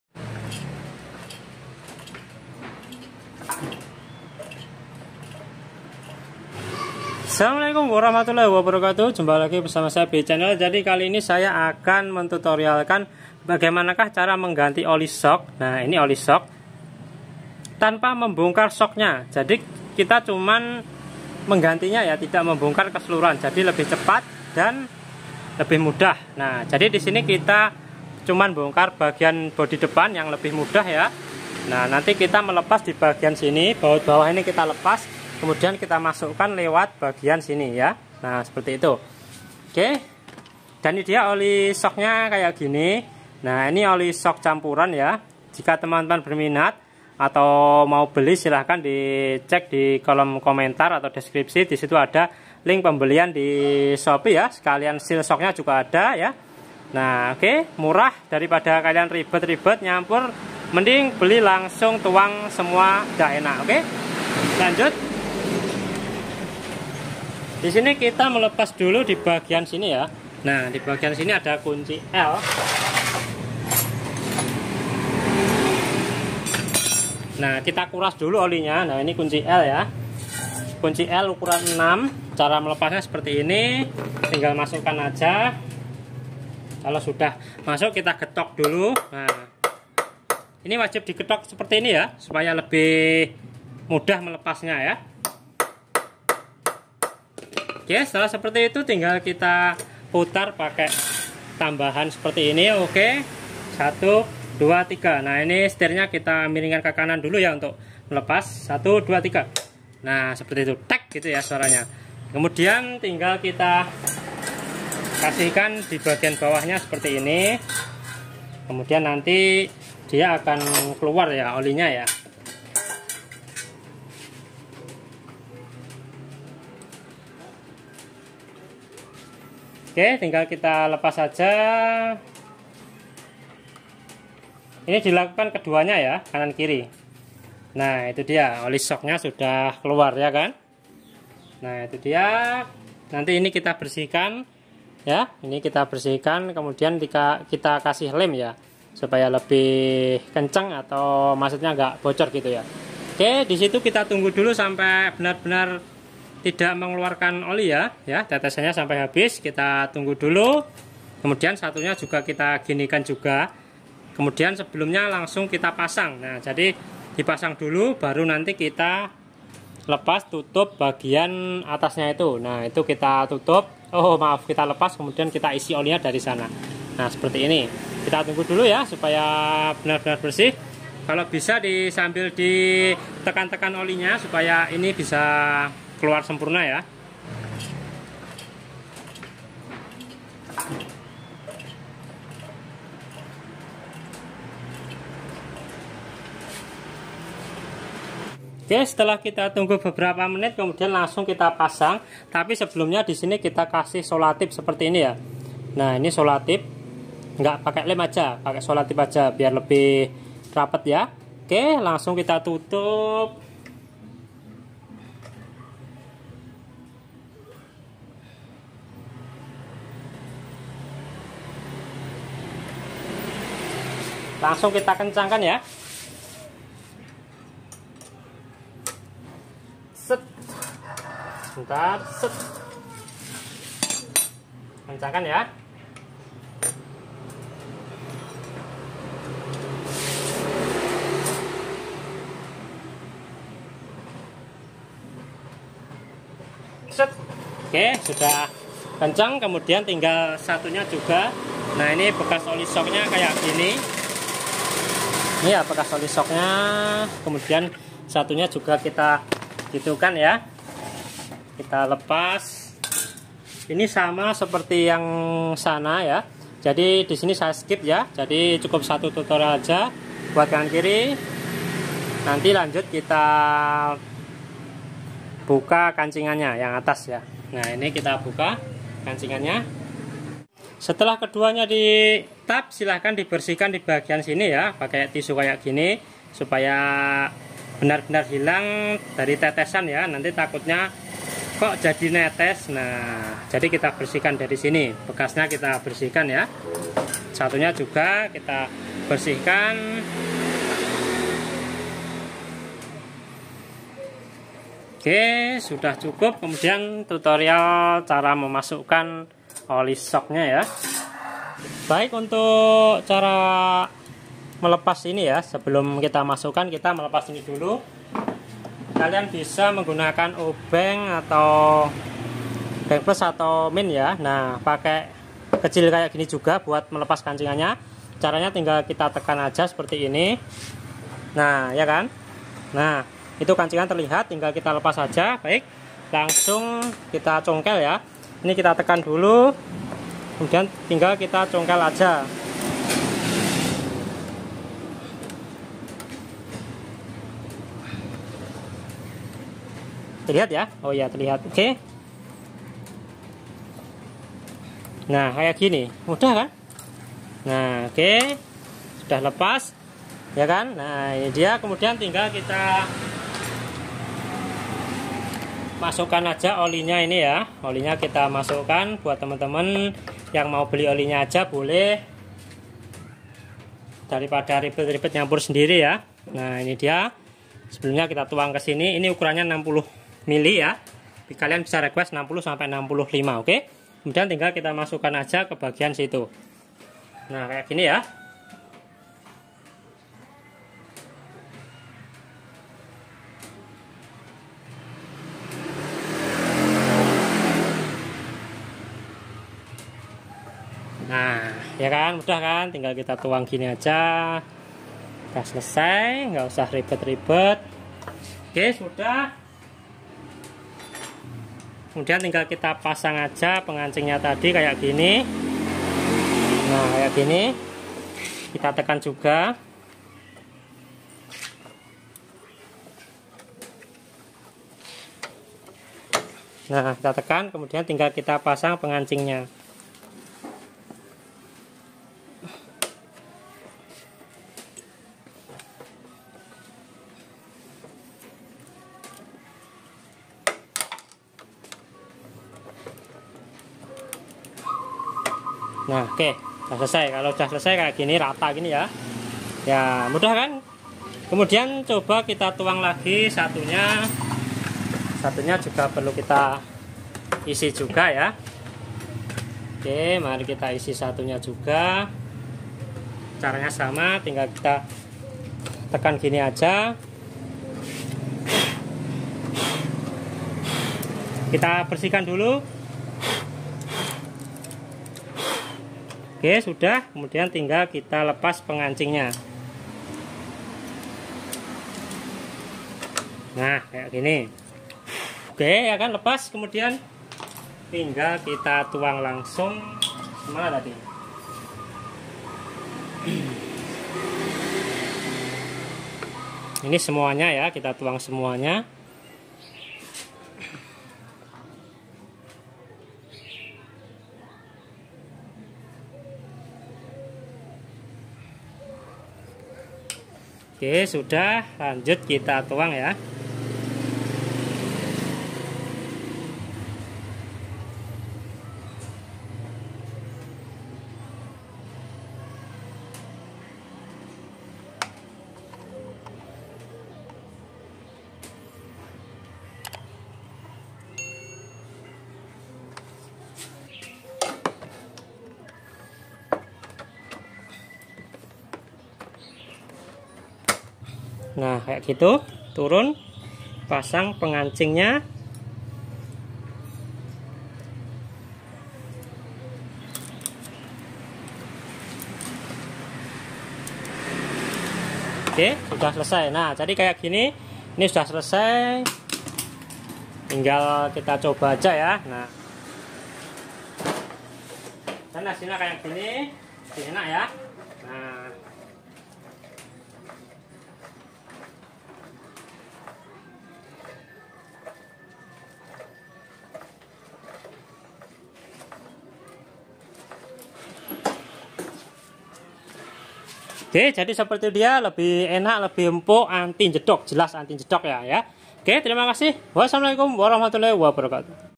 Cara mengganti oli shock tanpa bongkar shock nya !!
Cara mengganti oli shock depan tanpa membongkar shock-nya

Kali ini saya akan mem tutorialkan bagaimanakah cara untuk mengganti oli shock depan tanpa bongkar shock-nya, di sini kita menggunakan alat yaitu kunci l ukuran 6, 7 ukuran 8, obeng, dan obeng minus kecil.

Sebelumnya kalian harus membuka dulu badang bagian depan supaya bisa untuk melepas penutup shocknya untuk memasukkan oli shock-nya nanti.

Kemudian tinggal kita lepas saja baut bagian bawah kemudian kita TAP dan tinggal kita tunggu kemudian tinggal kita isi aja.

Begitulah mudahnya cara mengganti oli shock tanpa bongkar shocknya untuk lebih lengkapnya silakan ditonton videonya sampai selesai biar nggak tahu bagaimanakah cara untuk mengganti oli shock depan tanpa membongkar.